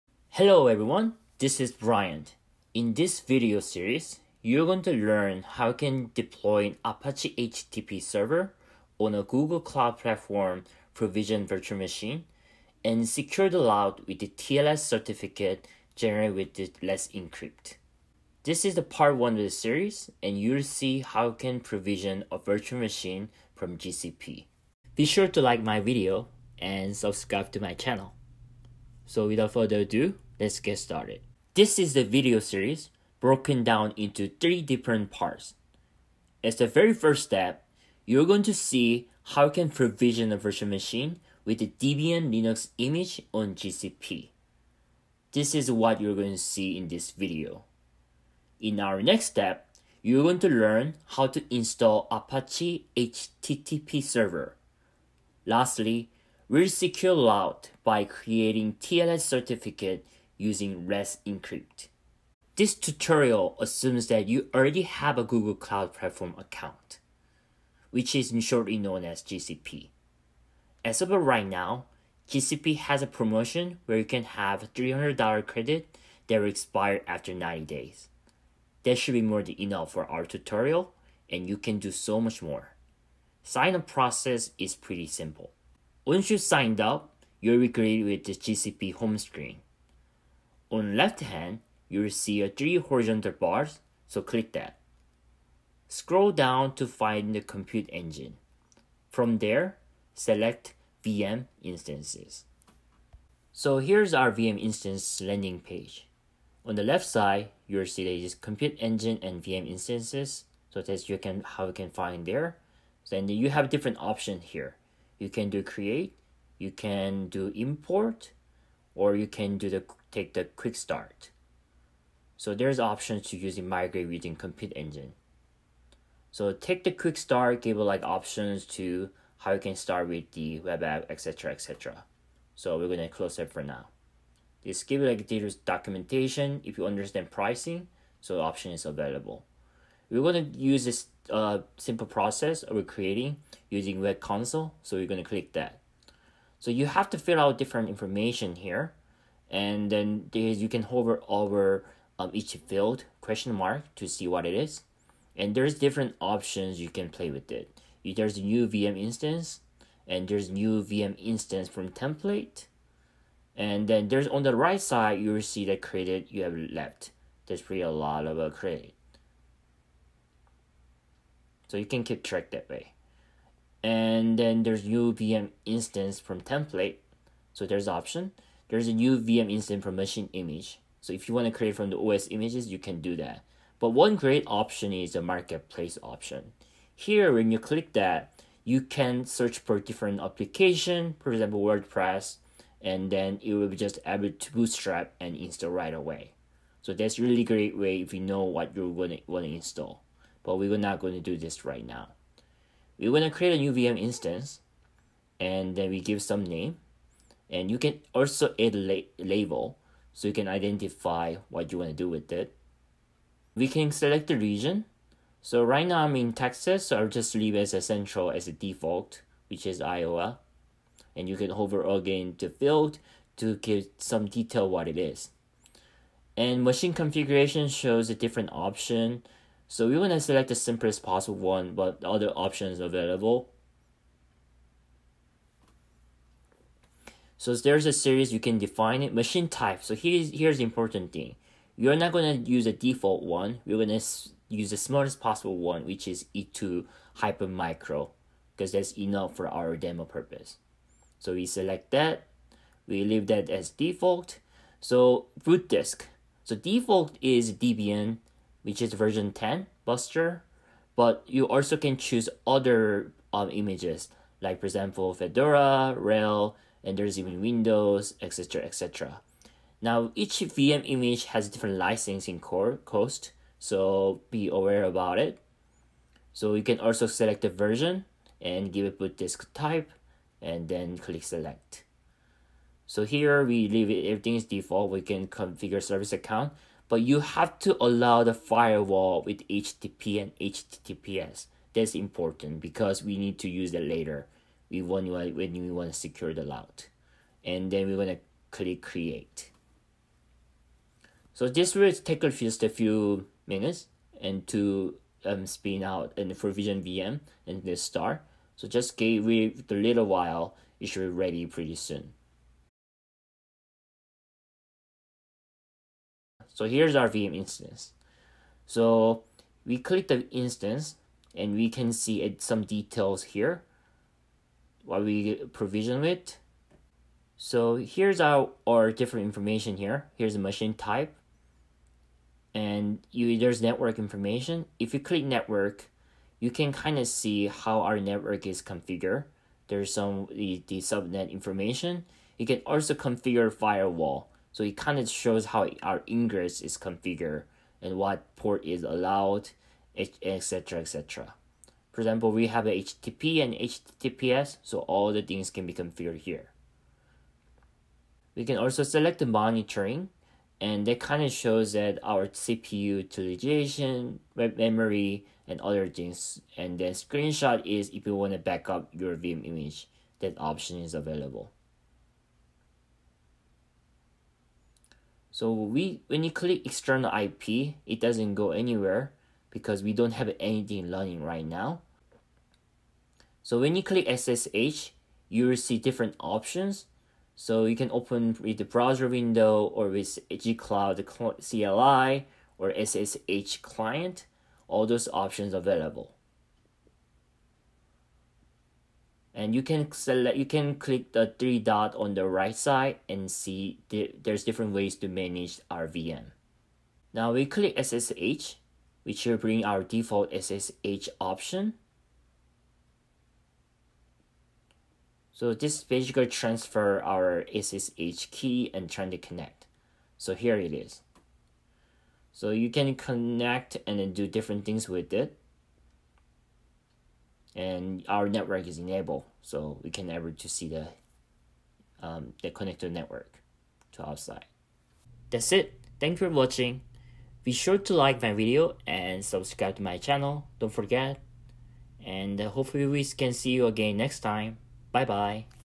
VM Running HTTPS Secured Apache Server on GCP - Part 1: Provision a Virtual Machine
In this video series, you are going to learn how you can deploy an Apache HTTP server on a Google Cloud Platform provisioned Virtual Machine and secure the route with TLS certificate generated with Let’s Encrypt.

This is part 1 of the series, and you will see how can you provision a Virtual Machine from GCP.